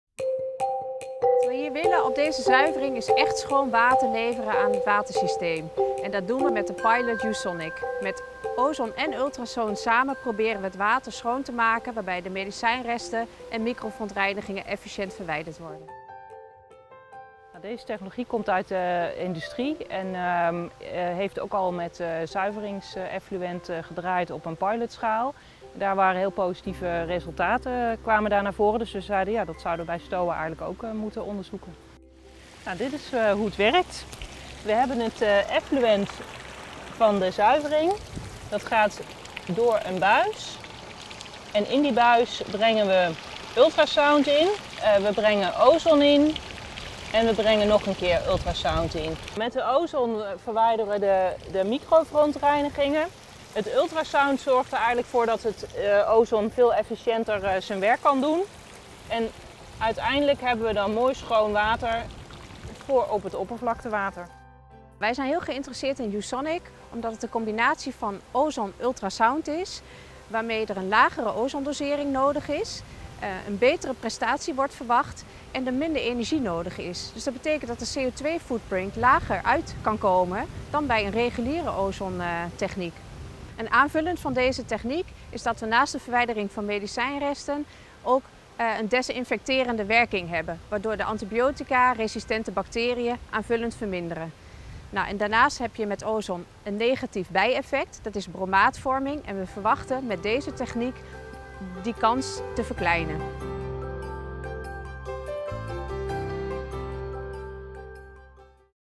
Waterkwaliteit | Verwijdering medicijnresten uit afvalwater, pilot ozon i.c.m. ultrasound | 2020
In deze korte video lichten vertegenwoordigers van waterschap Rijn & IJssel en Sweco een pilot toe waarin met behulp van ozon en ultrasound - Usonic - medicijnresten en andere microverontreinigingen uit het afvalwater worden verwijderd.  De pilot vindt plaats in het kader van het Innovatieprogramma Microverontreinigingen uit rwzi-afvalwater van STOWA.

Video afgespeeld tijdens het Waardevol Water Symposium op 16 oktober 2020.